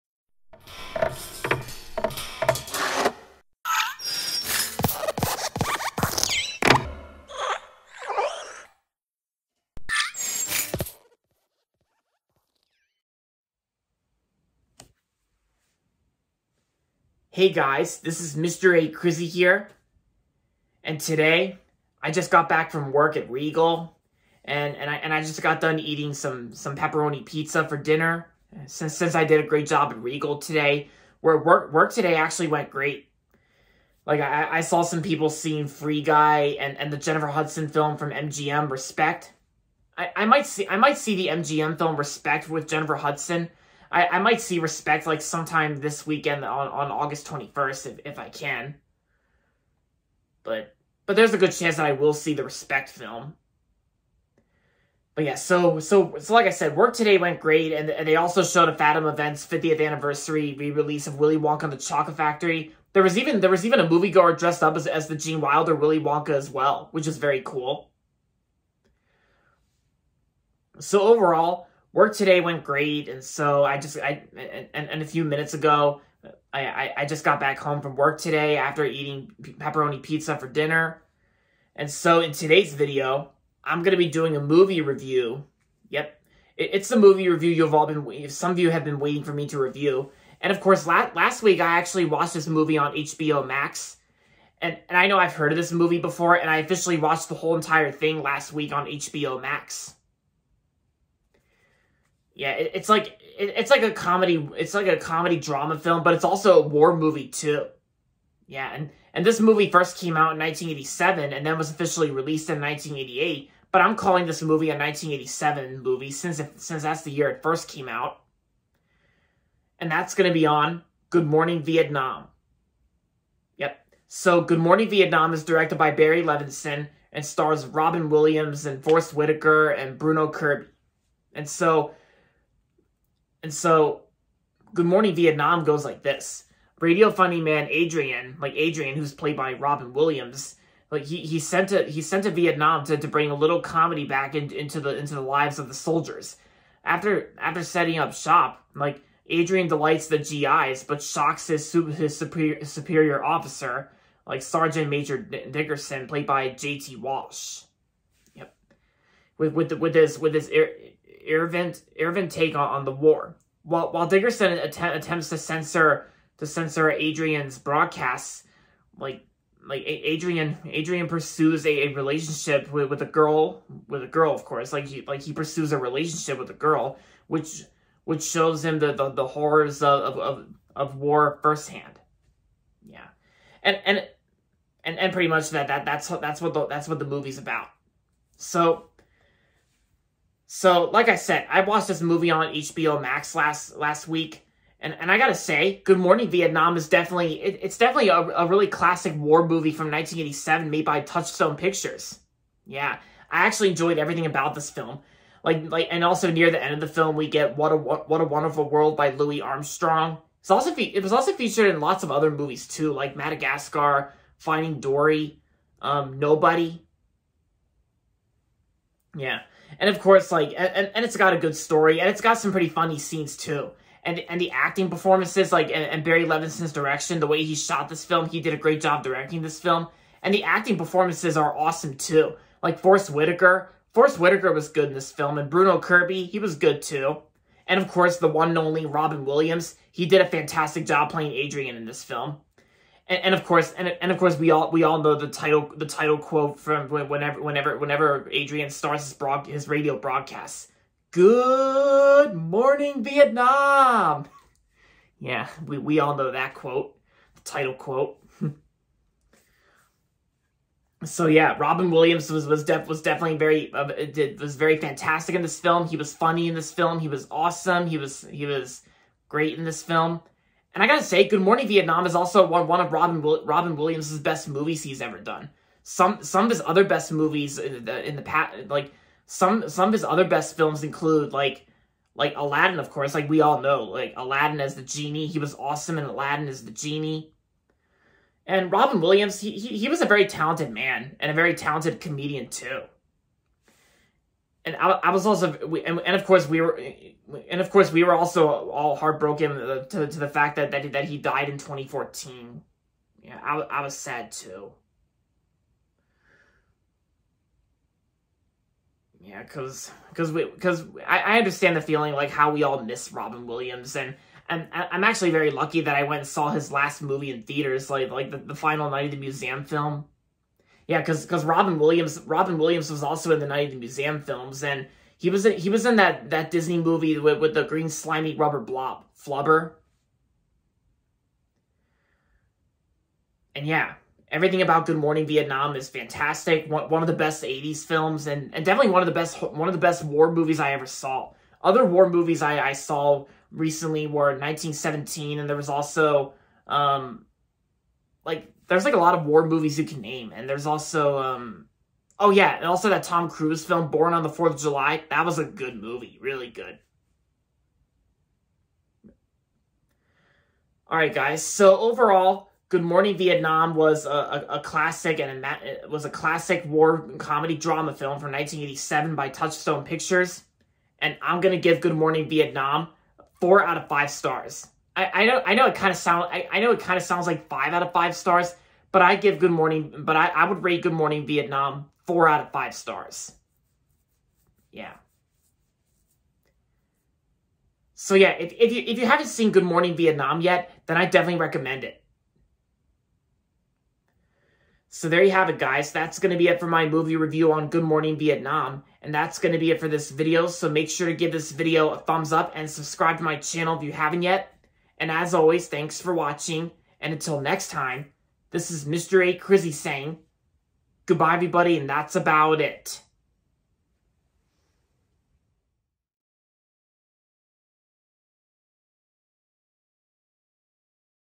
Good Morning, Vietnam (1987) Movie Review
Just got back from work today and it went really great. In today's movie review, I'll be reviewing a war comedy drama film I watched on HBO Max last week starring Robin Williams, Forest Whitaker, and Bruno Kirby. Good Morning, Vietnam.